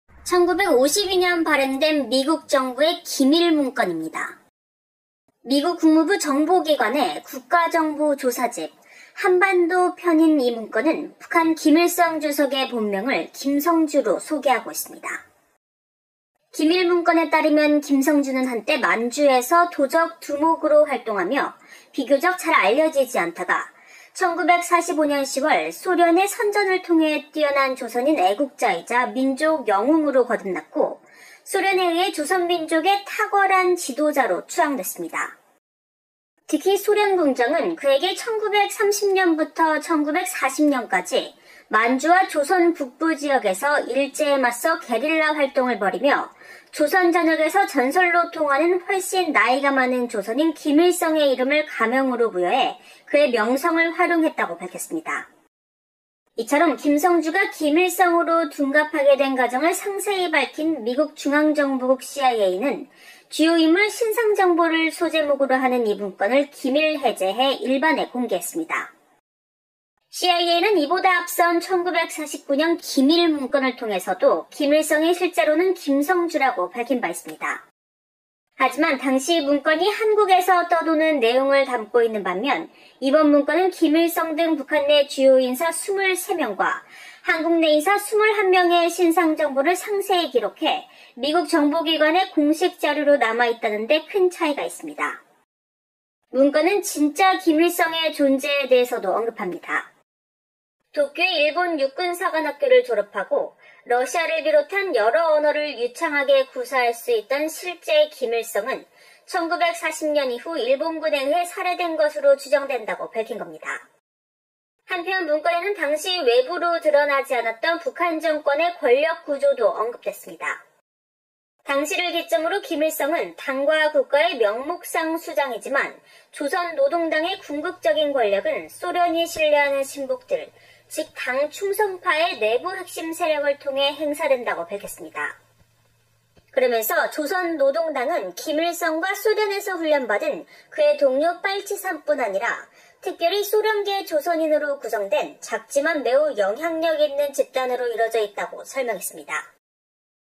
북한 김일성은 가짜다 1952년 CIA 기밀문서 일반공개!!
North Korea's Kim Il Sung is fake Kim Sung Ju's real name was released to the public in 1952 for confidential documents from the CIA!! 미국의 소리 방송보도내용